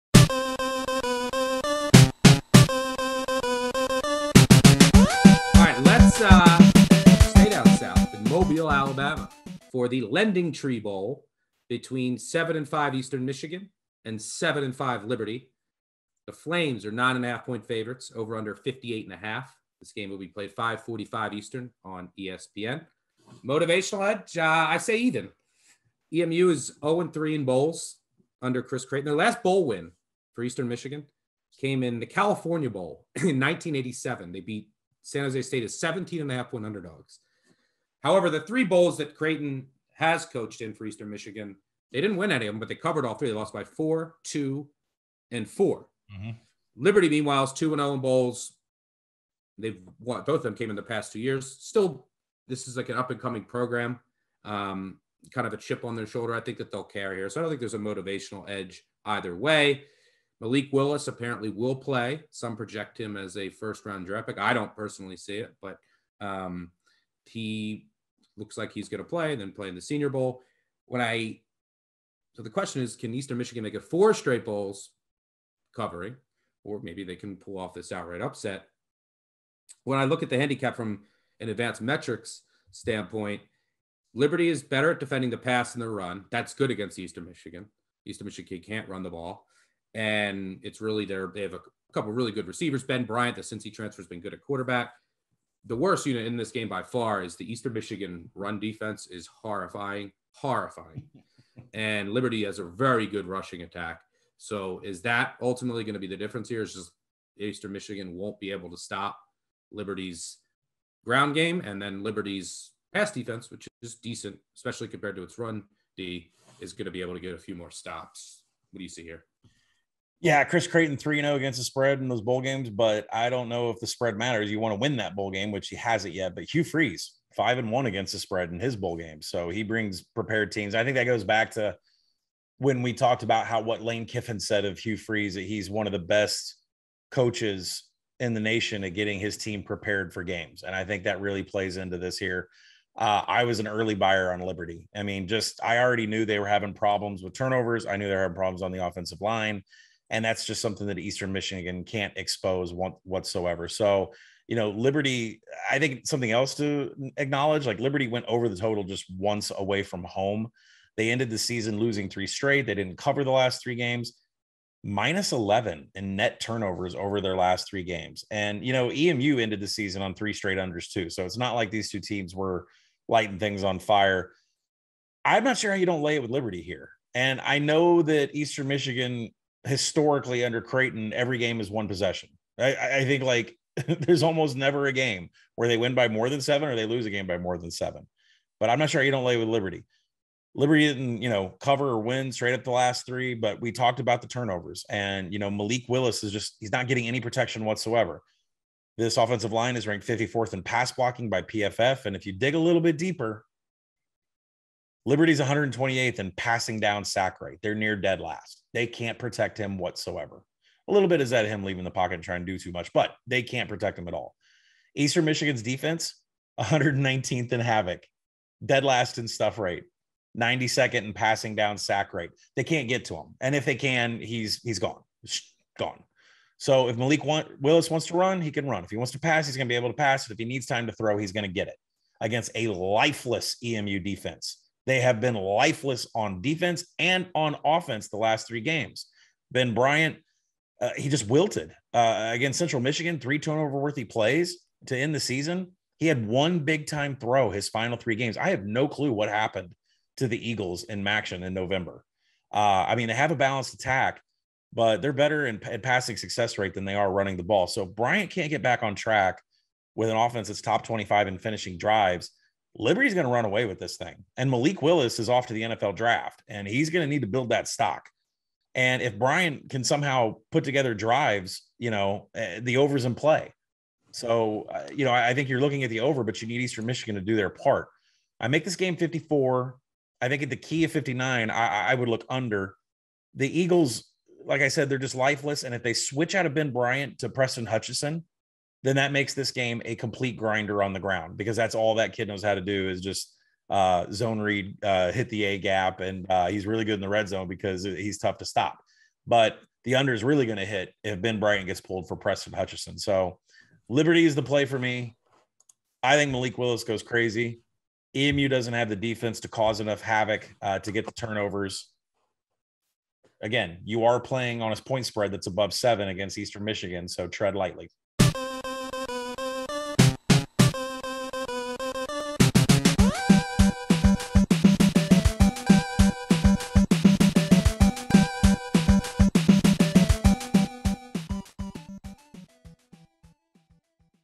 Lending Tree Bowl Game Picks 2021 | Eastern Michigan vs Liberty University
CFB Bowl Game Bets for the Lending Tree Bowl from Big Bets on Campus podcast hosts Stuckey and Collin Wilson. Tune in to hear how Stuckey and Collin are betting on Saturday's Lending Tree Bowl game between Eastern Michigan and Liberty University.